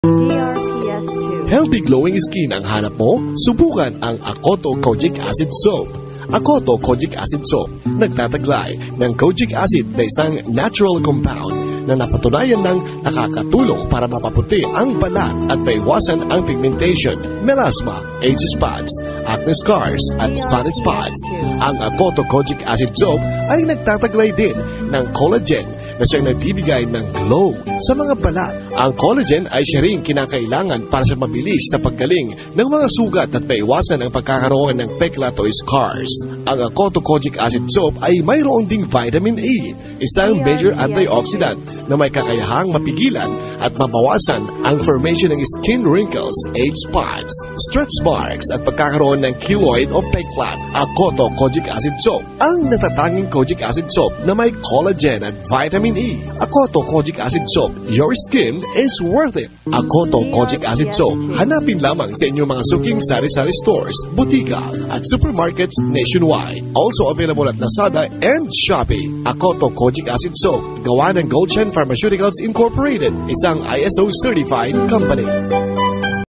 Akoto Kojic Acid Soap Radio Ad 2022 76s
This is the Akoto Kojic Acid Soap Radio Commercial from 2022. The said Kojic Soap was based from the makers of Jimm's Coffee Mix Brand.

DRPS Two 2280: Akoto Kojic Acid Soap Radio Ad 2022 76s